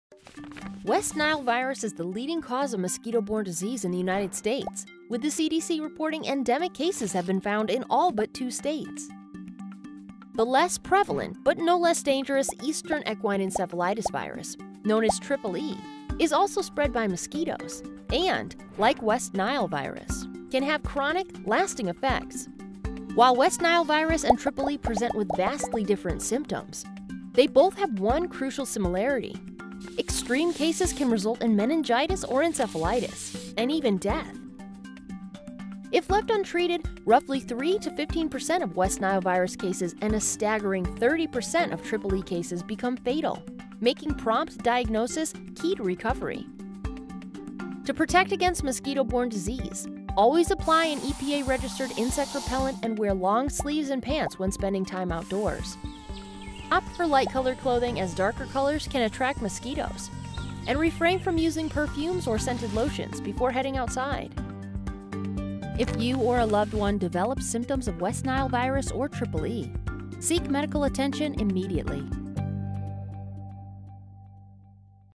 Medical Minute: West Nile Virus & Eastern Equine Encephalitis (EEE)
West Nile Virus and Eastern Equine Encephalitis, also known as "Triple E," are diseases spread by mosquitoes. While they can both cause serious, chronic effects, each presents with vastly different symptoms. The one similarity? Extreme cases of both can result in Meningitis or Encephalitis, and even death.

Learn more about mosquito threats and prevention tips at PestWorld.org